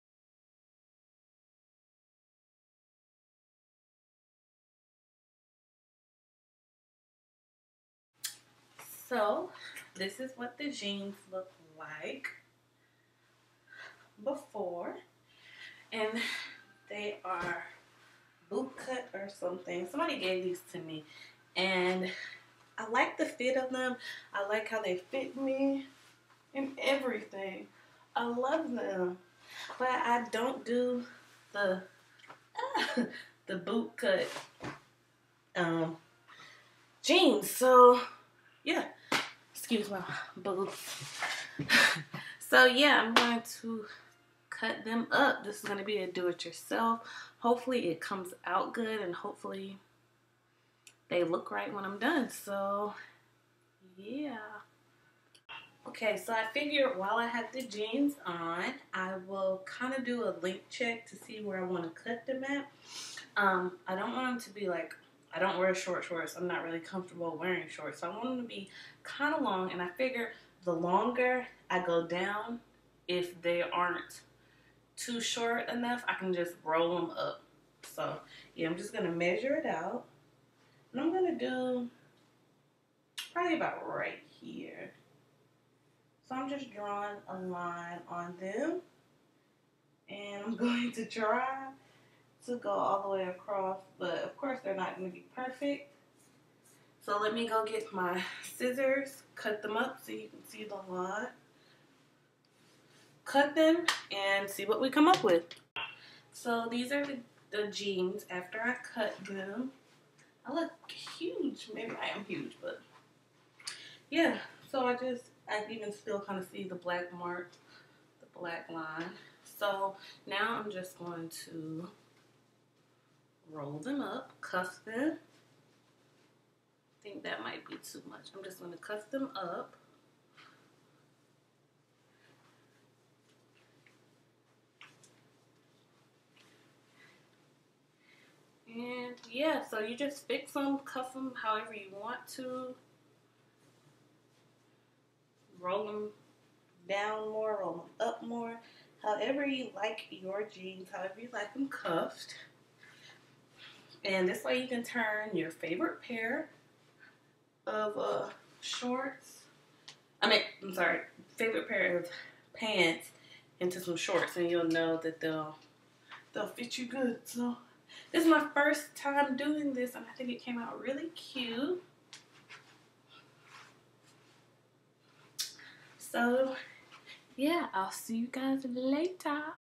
DIY Shorts from those UGLY bell bottom jeans
Hey yall its Jasmine again and this time I am doing a DIY (do it yourself) craft in which I used some jeans that my step sister gave me and just cut them into shorts and rolled them up. This would be good to do if you have a pair of jeans that done really fit how you like in the bottom just cut them up and see what you come up with ! I hope you enjoy!

PS. I purchased all products :)